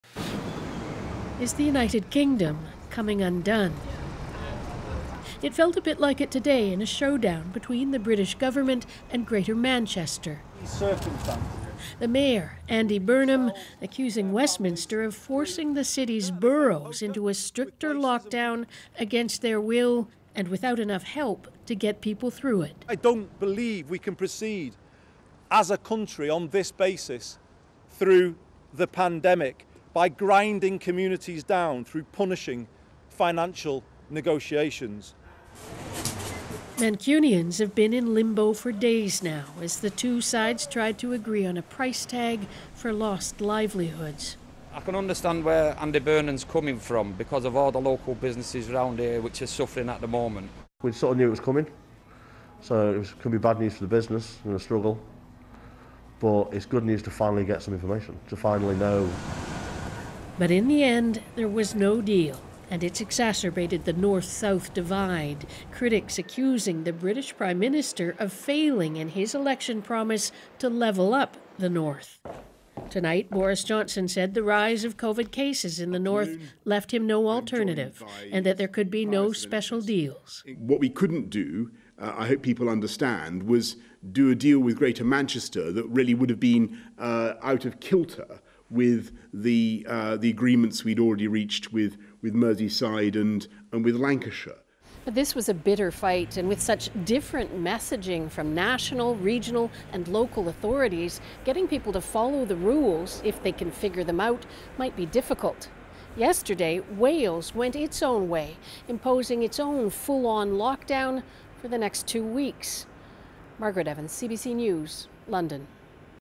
Lockdown measures in Manchester met with criticism
As COVID-19 cases surge in Britain, Prime Minister Boris Johnson announced the highest level of pandemic restrictions in the country’s second biggest city, Manchester. Manchester's mayor says he feels abandoned without adequate financial support for his residents.